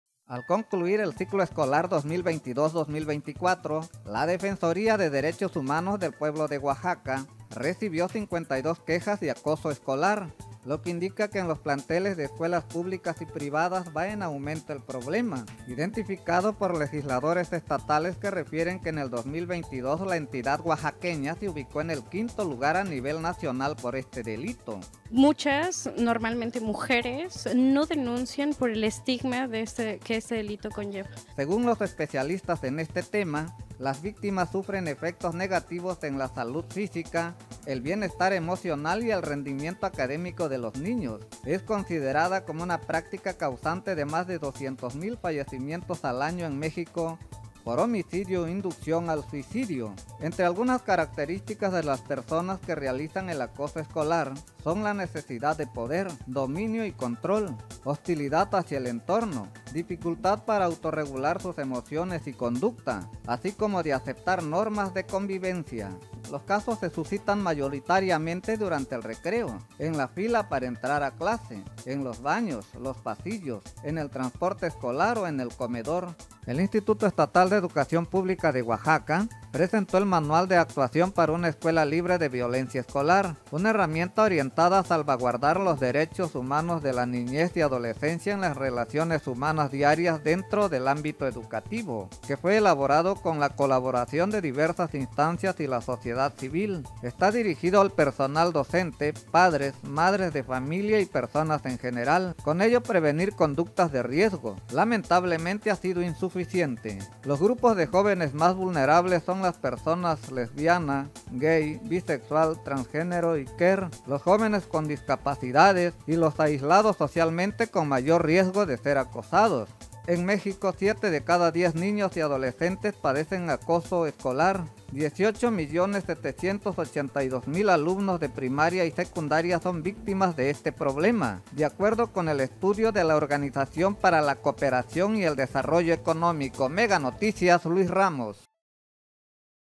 NP ACOSO ESCOLAR, OAXACA
Oaxaca ocupa el quinto lugar nacional de acoso escolar.